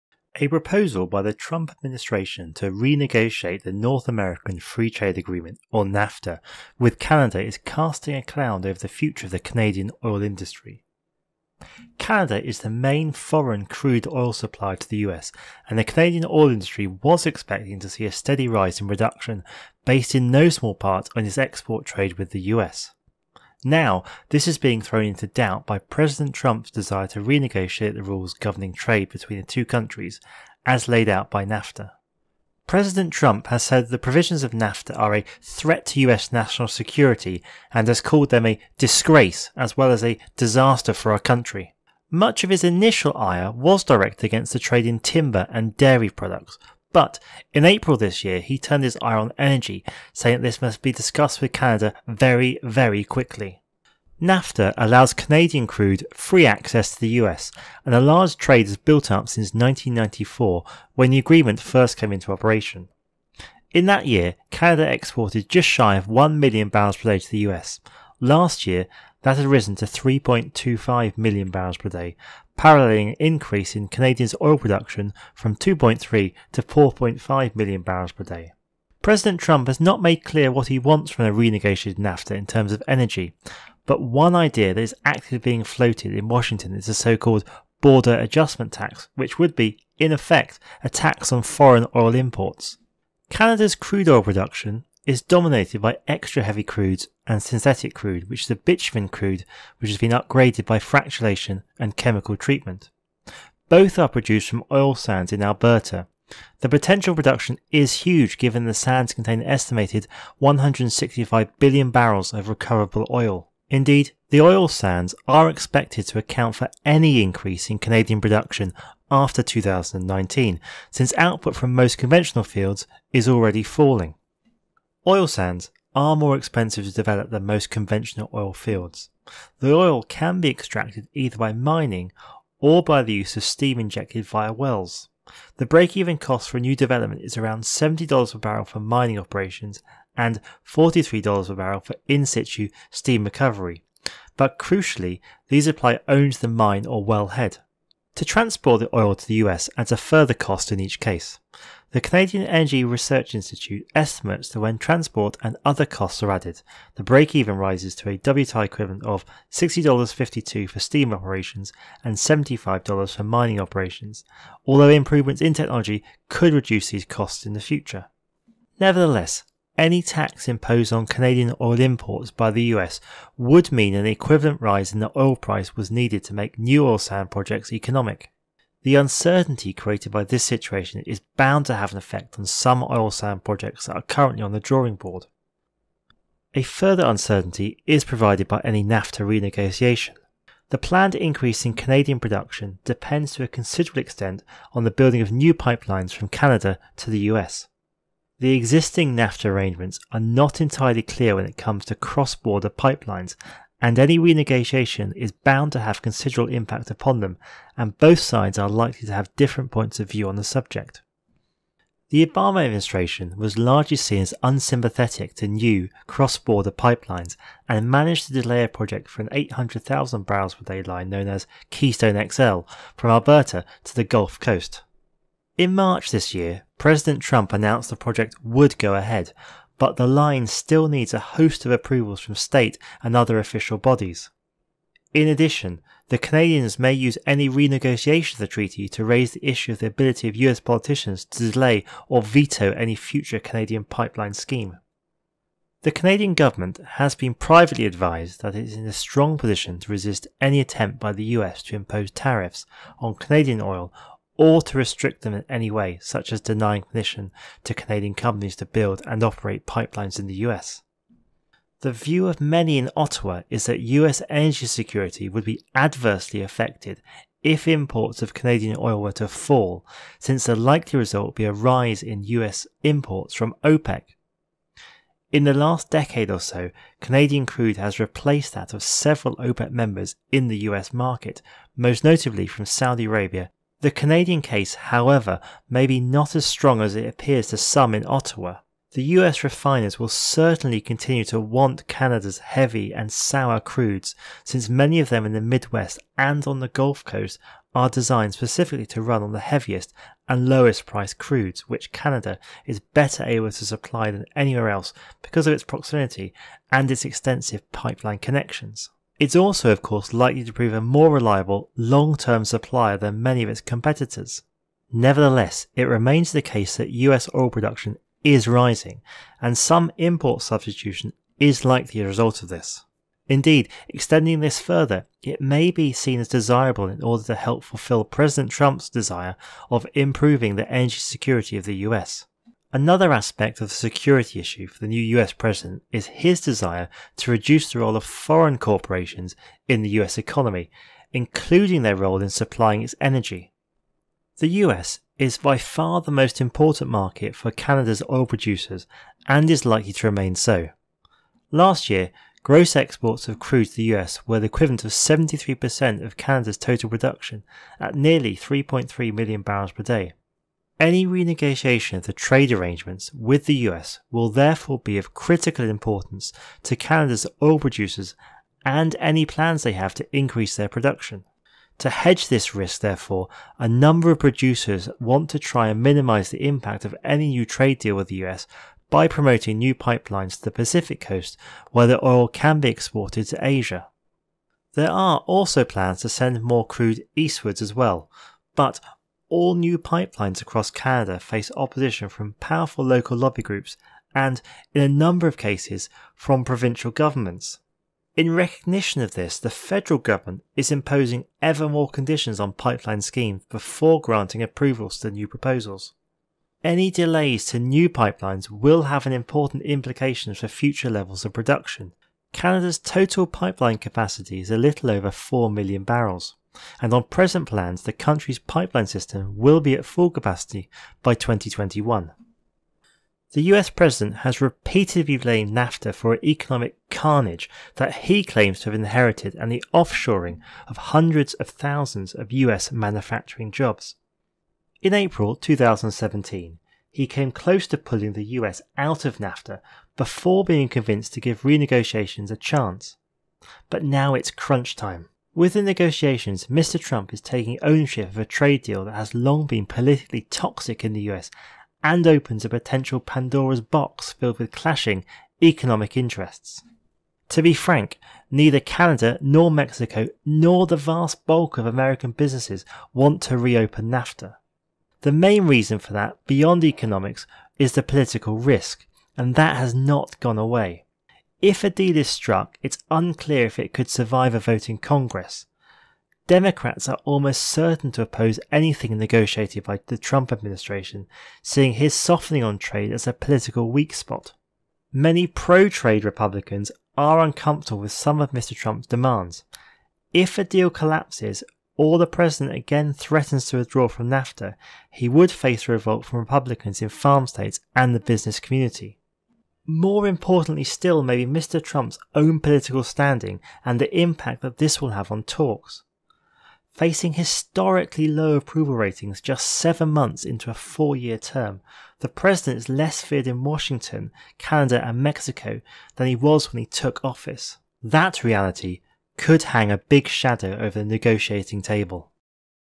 NAFTA and the Fight for Canadian Oil
A proposal by the Trump administration to renegotiate the North American Free Trade Agreement (NAFTA) with Canada is casting a cloud over the future of the Canadian oil industry.

NAFTA allows Canadian crude free access to the US, and a large trade has built-up since 1994 when the agreement first came into operation but what will be the impact of a NAFTA renegotiation on the Canadian Oil industry?